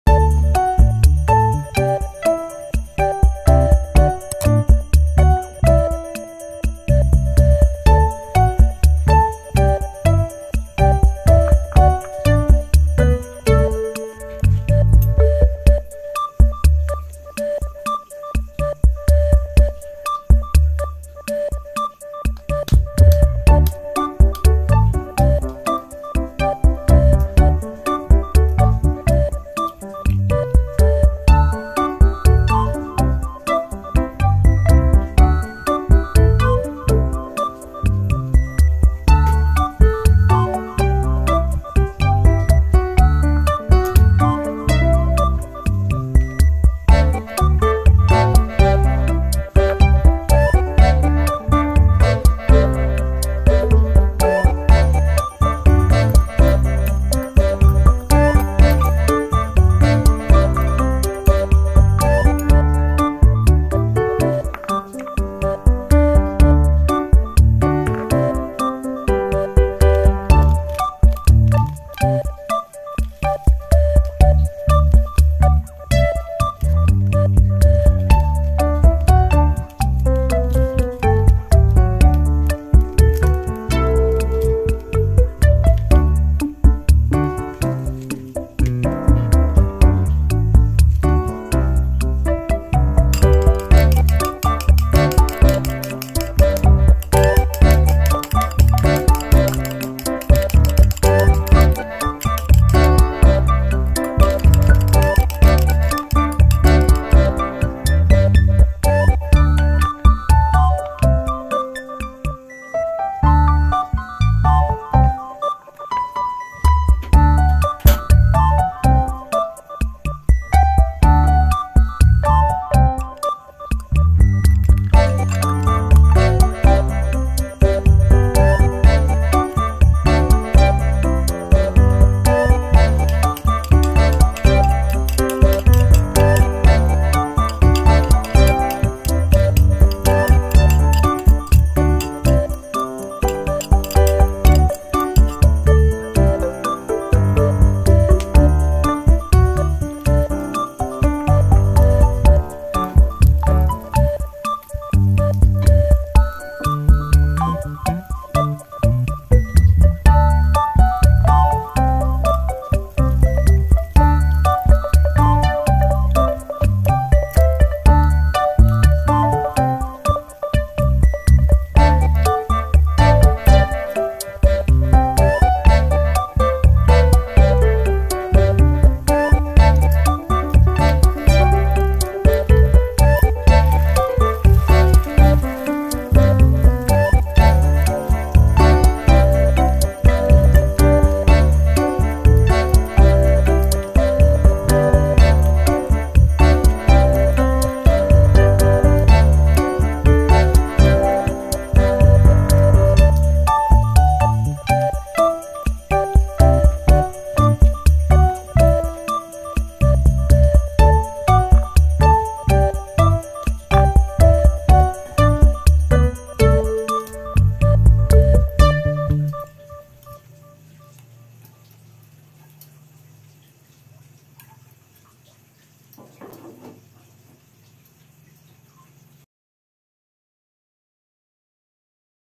TANGGAL TUA⁉️ MASAK TELUR DADAR SOLUSINYA 🤤😋
Assalamualaikum teman teman pada video kali ini aku akan membahas "Tanggal Tua⁉️ Telur dadar solusinya" langsung cuss ke video :)
Yukkk tonton juga